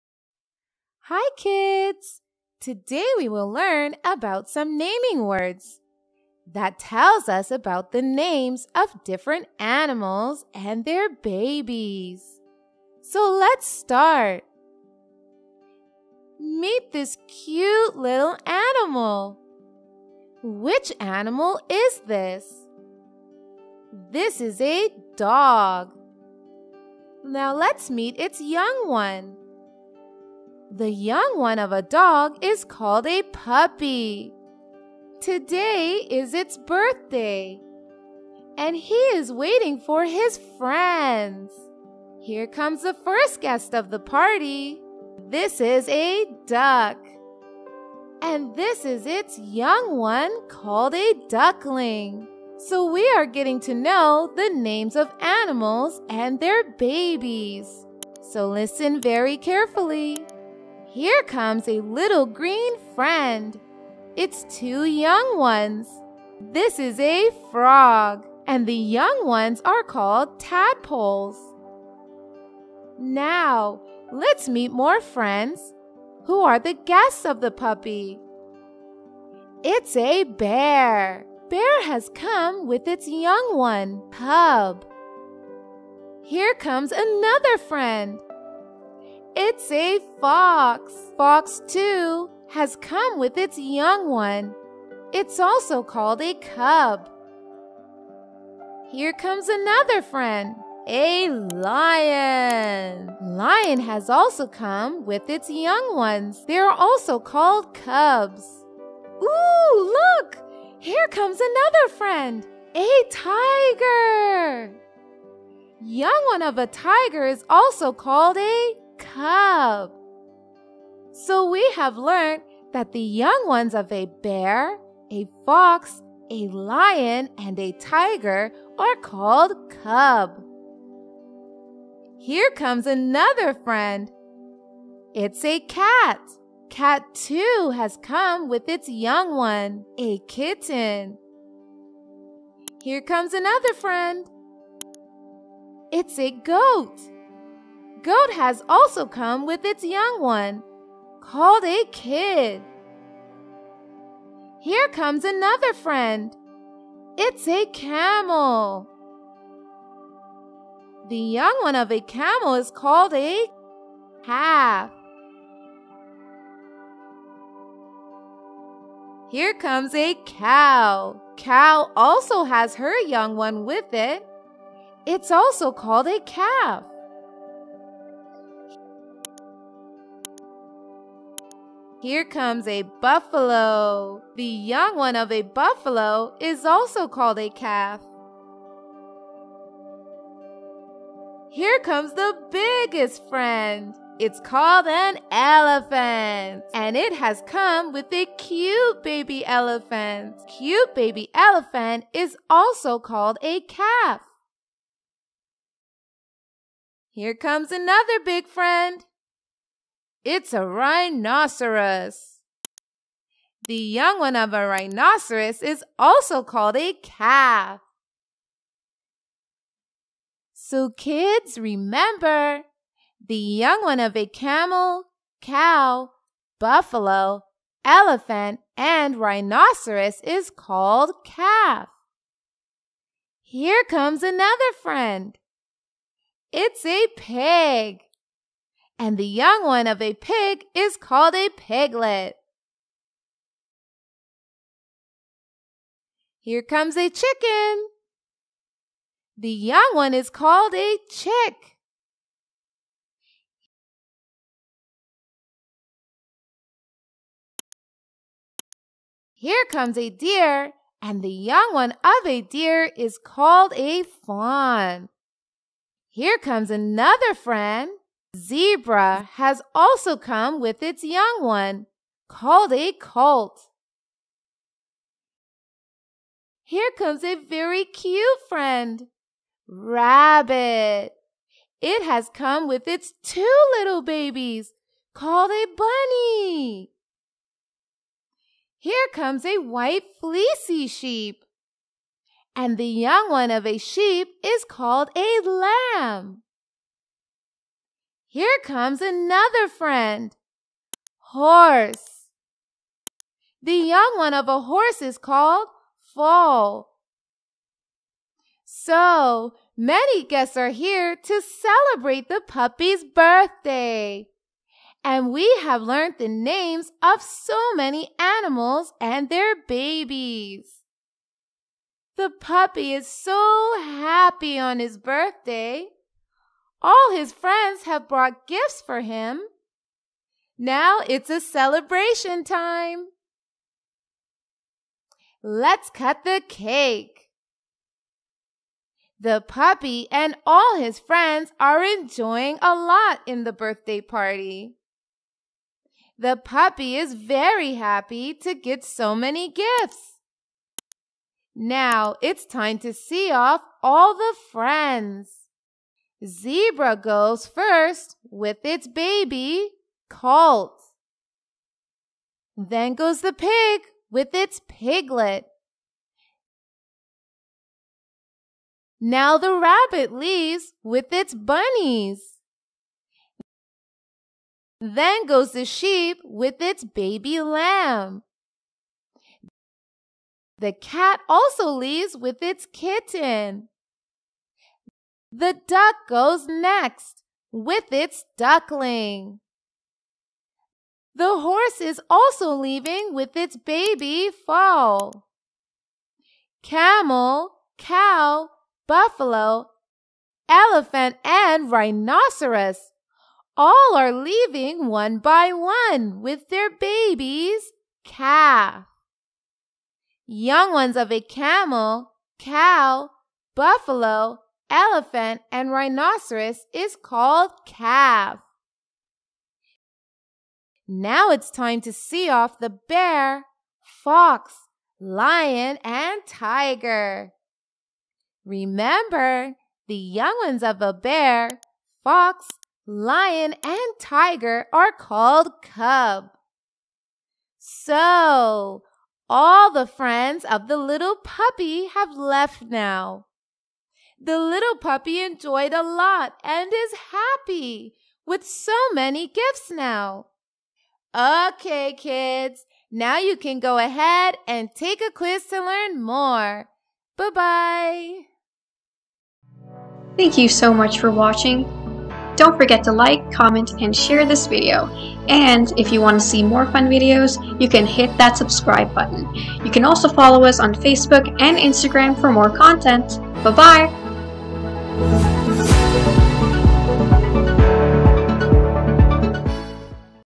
Animals and their Young Ones | Animals & their Babies | Baby animal | Science
Naming words of animals and their babies

This educational video explains about animals and their young ones. This will help them a lot in their daily routine.